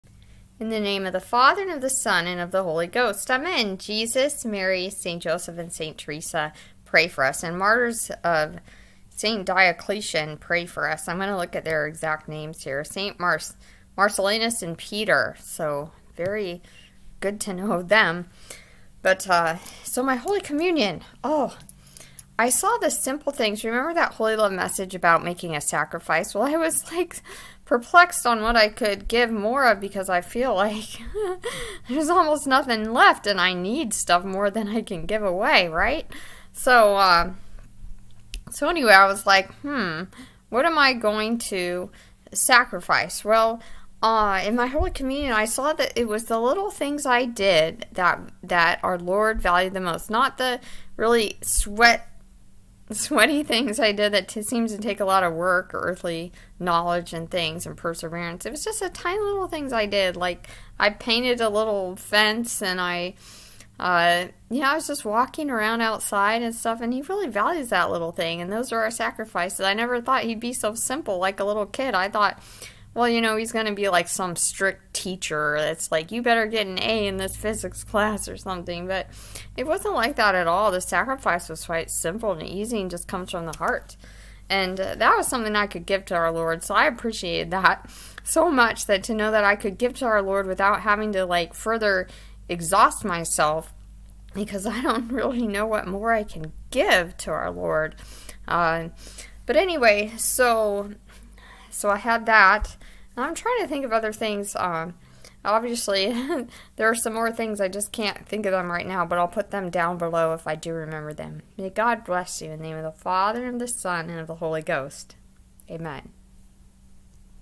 Anna's Holy Communion; it's the simple things we sacrifice that mean the most to God.
After reading the latest HolyLove.org message I thought that I would need to try to sacrifice more, and I can't think of anything more I can do. Well, God showed me that it's the little simple things I do that matter to Him the most. He's not like us and does not want or need a vain show of earthly power, prestige, wealth and success. Our Lord was happy that I painted a little fence on a birdhouse and took a walk on a sunny day.
I also saw how Happy Our Lord was with the little prayers I did in the middle of the night. I had no idea Our Lord valued so much my little rosaries in the middle of the night. He likes it when we can't sleep and think of Him, and pray like the nuns and monks do the office in the middle of the night. He like that very much!  All the simple, little things we do He appreciates more than the great things we "think" we are doing "so well, so "great"."